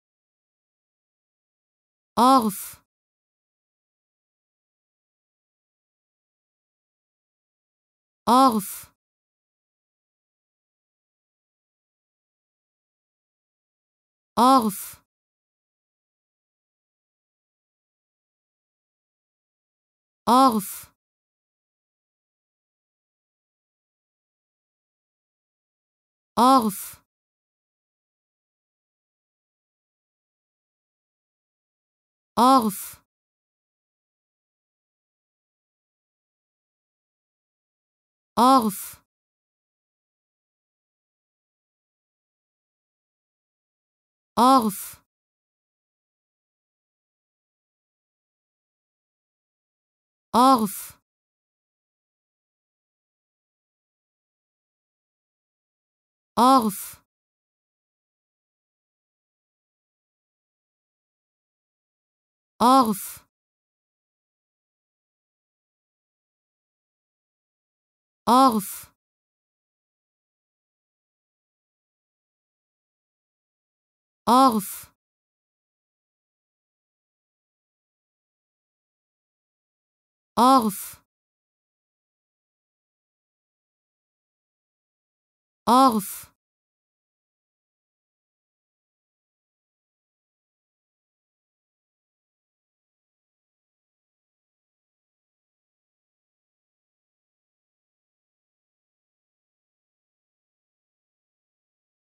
How to Pronounce ''Orff'' Correctly in German
Learn how to say ''Orff'' in German with this short pronunciation tutorial.